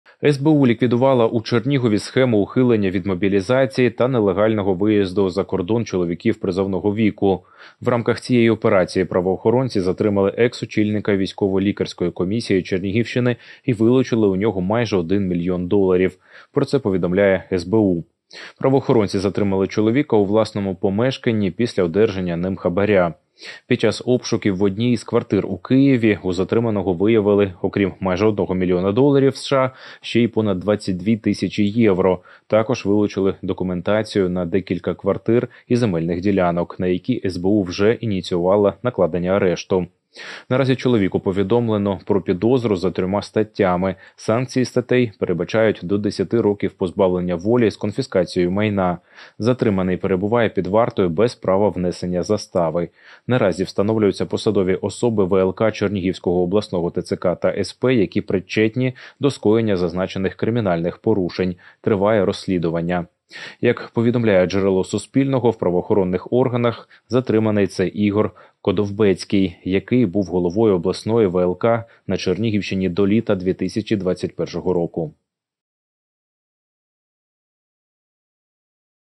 Під час обшуків у колишнього голови Чернігівської обласної ВЛК виявили майже 1 мільйон доларів США
Під час обшуків у колишнього голови Чернігівської обласної ВЛК в одній з квартир у Києві виявили майже 1 мільйон доларів США.